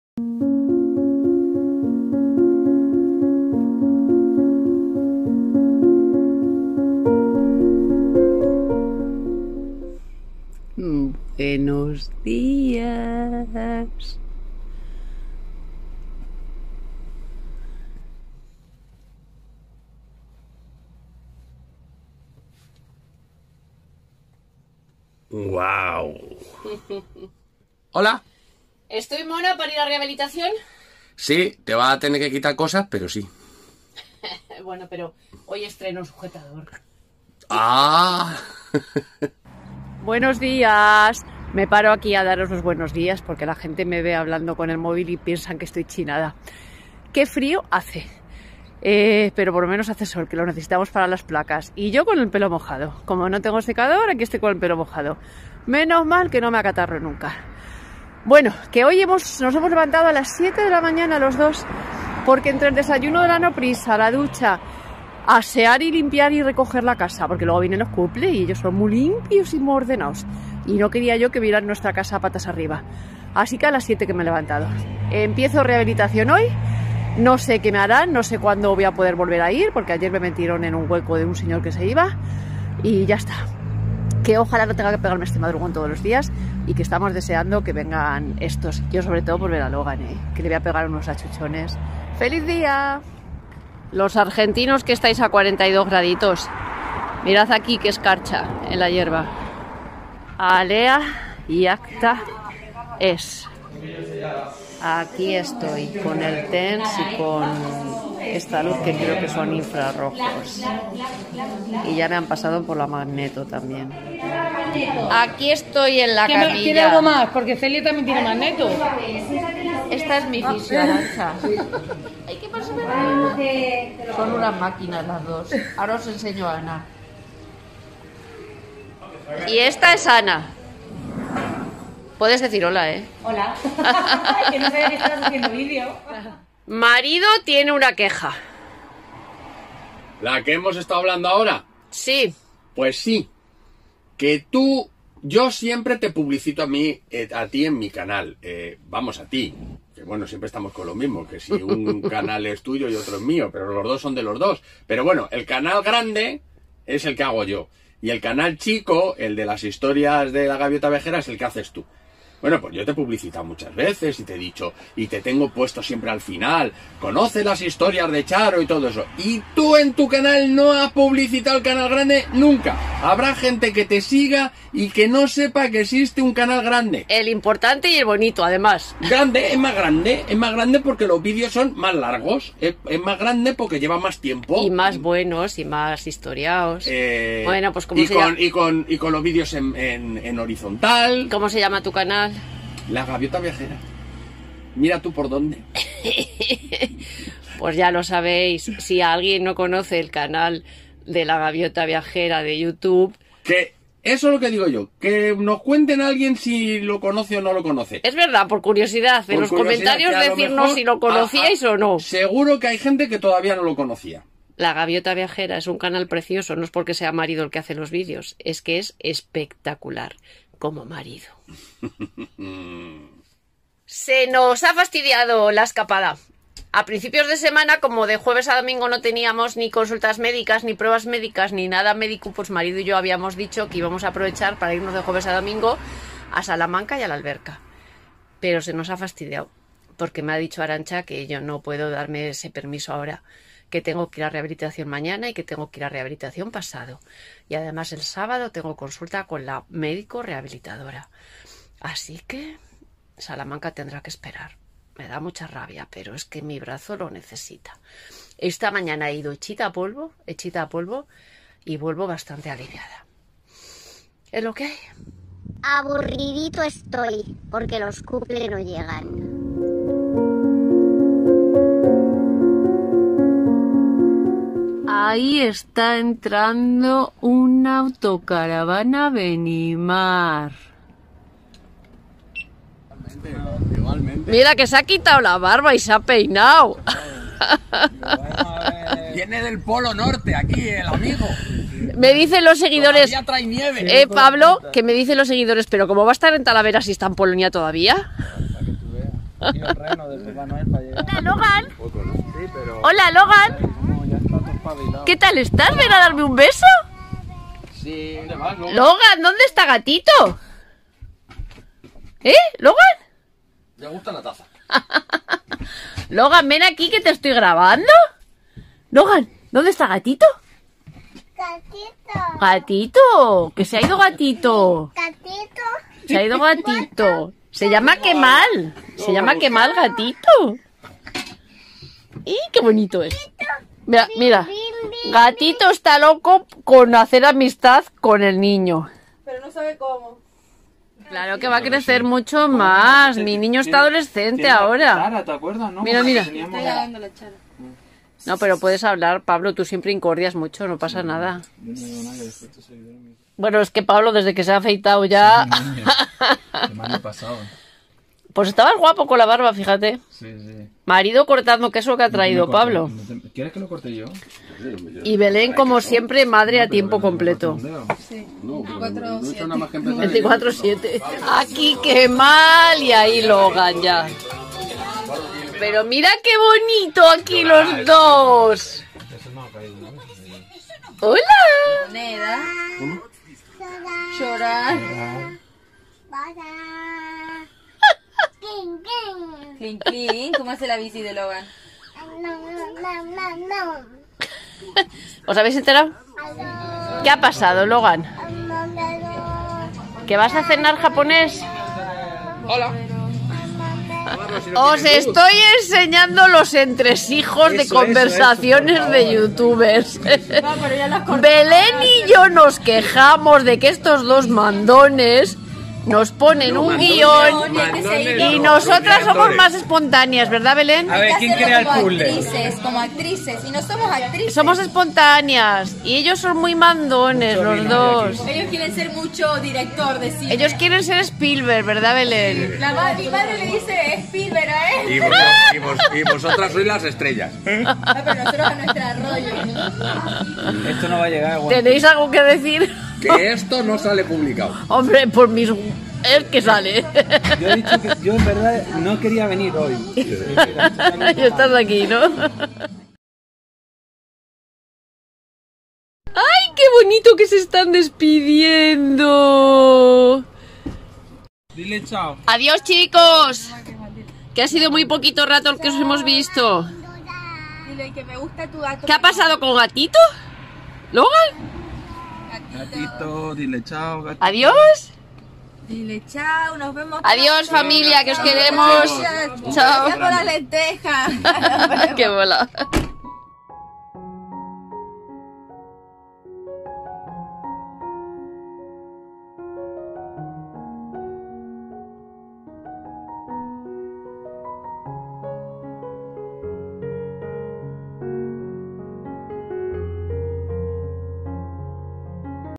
MARIDO tiene una QUEJA Enero 2022 #304
Seguimos de médicos y vienen a visitarnos la familia Cuple a Talavera.